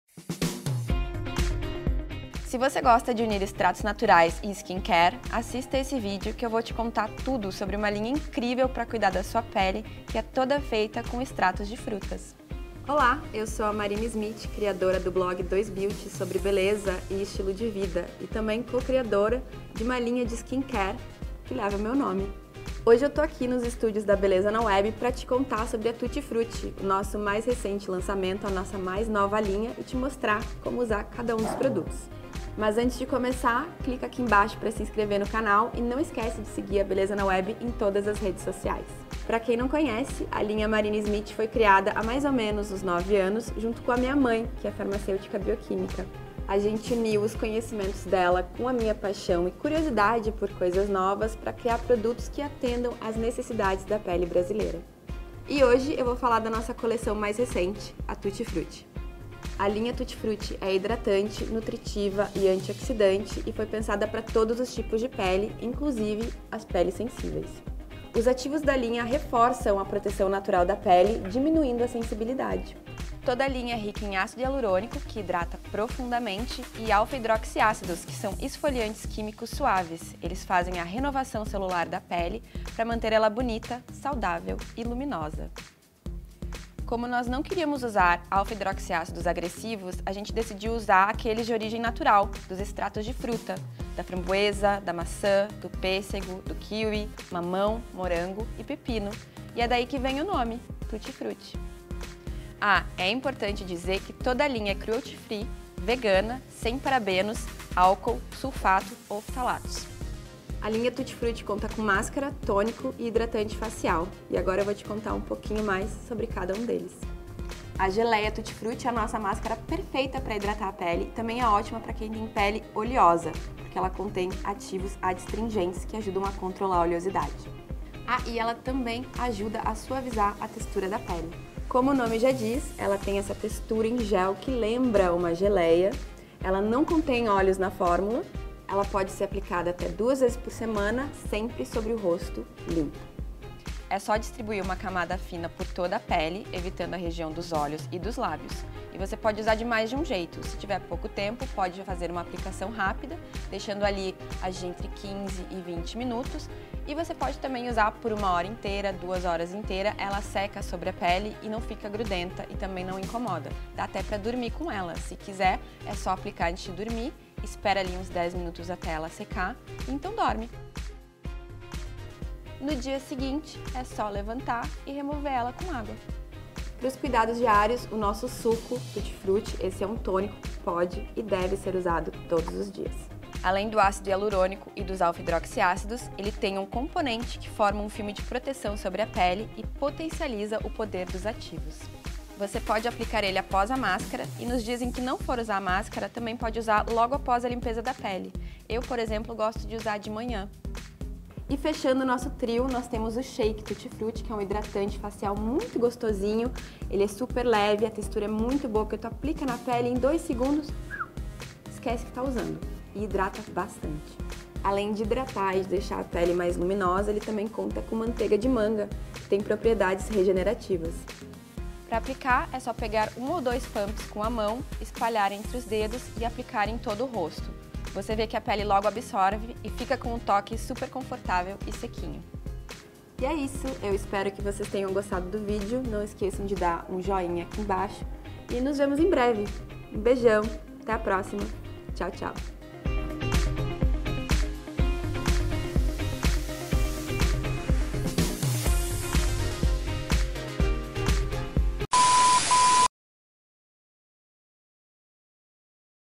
Linha Tutti Frutti por Marina Smith I Beleza na Web
Já imaginou uma linha de skincare com AHAs de origem natural?

Marina Smith apresenta a linha de skincare Tutti Frutti: máscara facial, tônico e hidratante facial que trazem luminosidade, renovação celular, nutrição e suavidade para todos os tipos de peles, até as mais sensíveis. Com ácido hialurônico e alfa hidroxiácidos de origem natural (de frutas como maçã, morango, framboesa, kiwi, mamão e pepino), a linha Tutti Frutti ainda conta com produtos de texturas únicos, fáceis de aplicar e muito práticos. Quer conhecer melhor? É só dar play.